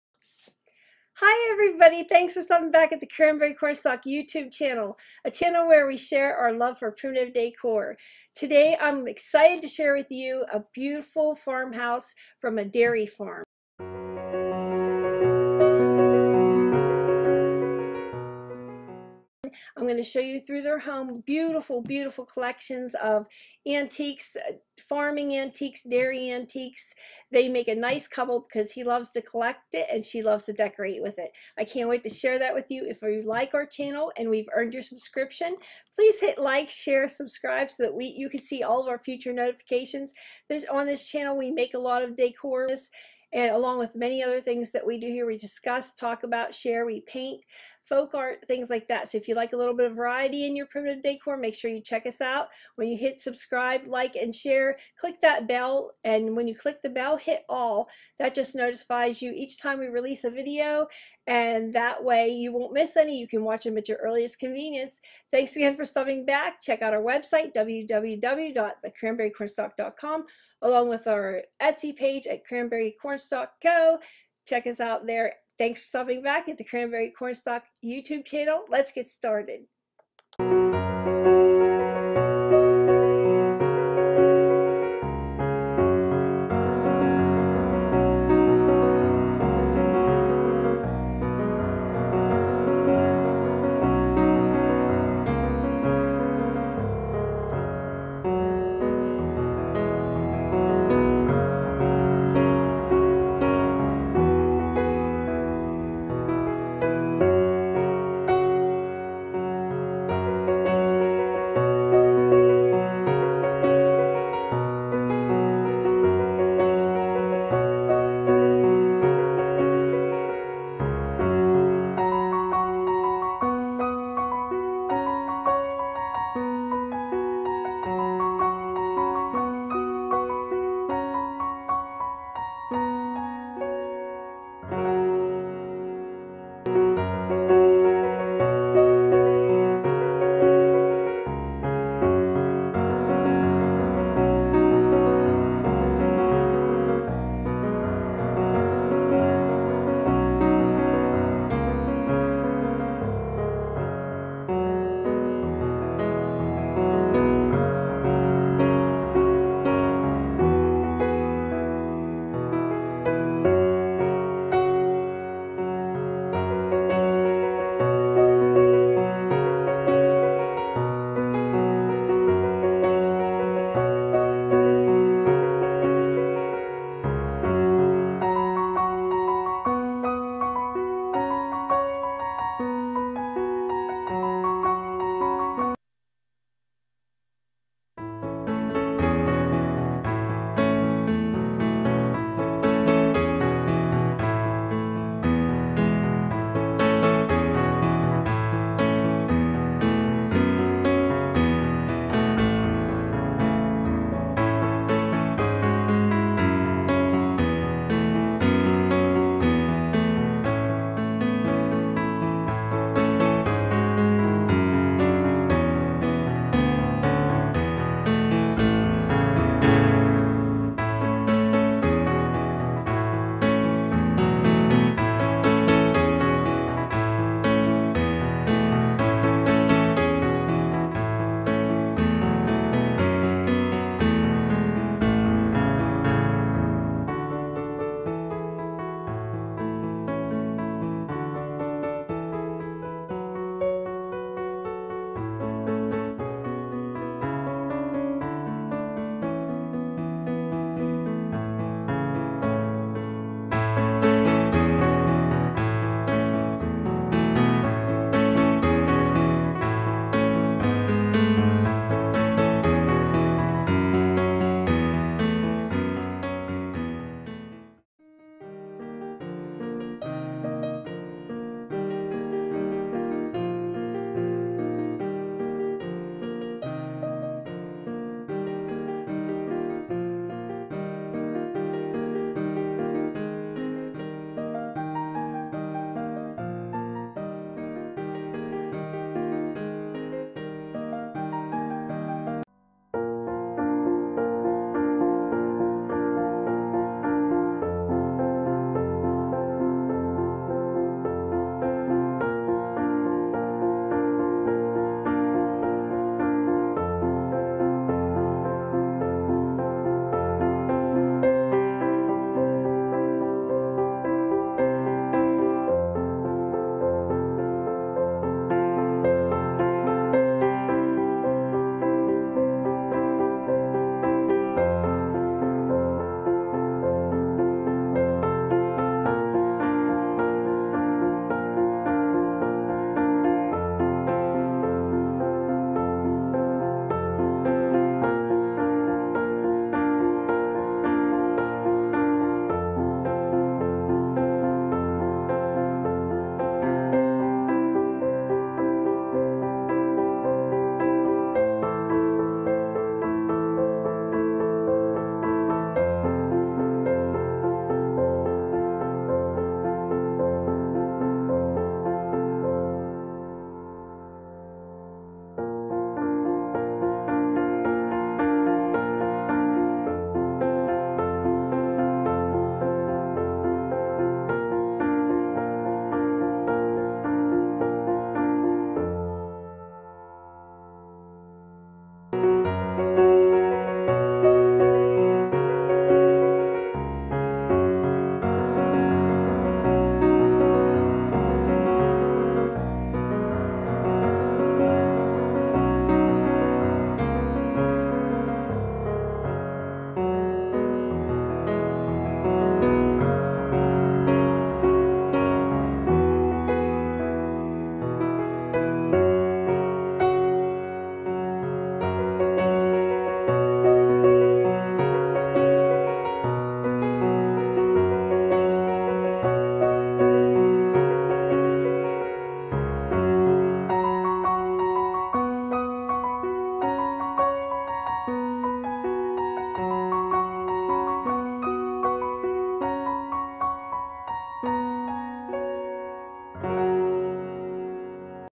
Colonial Primitive Decor Maggie&Leland 2
Another beautiful Colonial Primitive home tour to end and begin the new Year! TheCranberryCornstalk.com